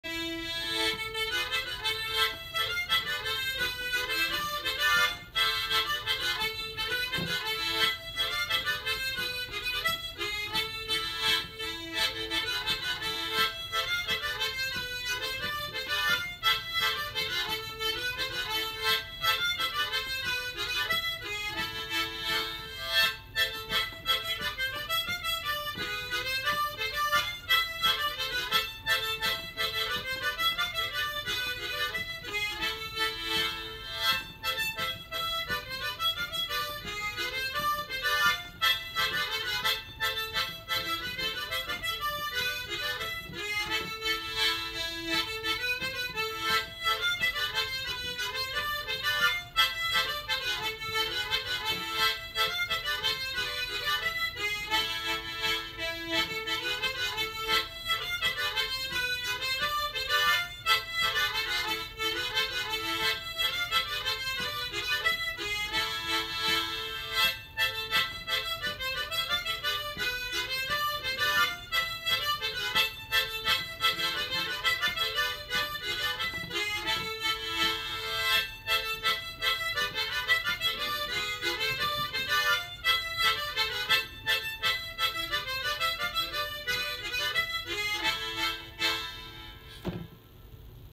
Jenkka, A-minor. Huuliharppu, Kielibasso, trad., mundharp, mundharmonika, tremolo harmonica
Jenkka, A-minor

A tune that I have made up

Huuliharppu
Kielibasso
trad.
mundharp
mundharmonika
Suzuki’s ‘Humming-mate’ tremolo harmonica